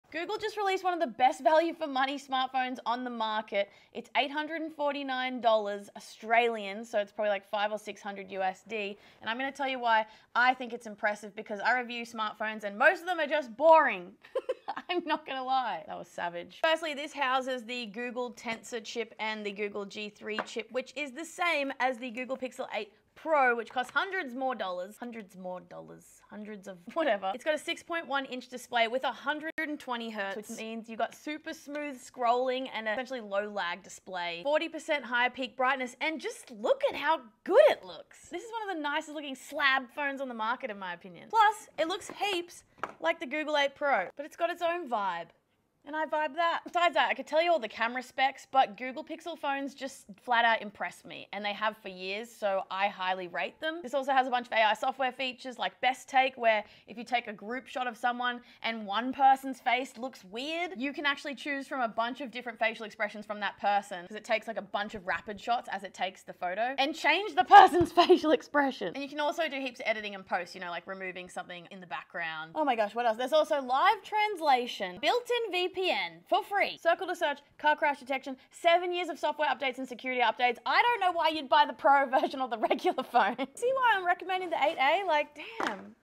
Google Pixel 8a is the best value smartphone of 2024
The Google Pixel 8a is, so far, the best value smartphone of 2024 and here's why. I think it's awesome!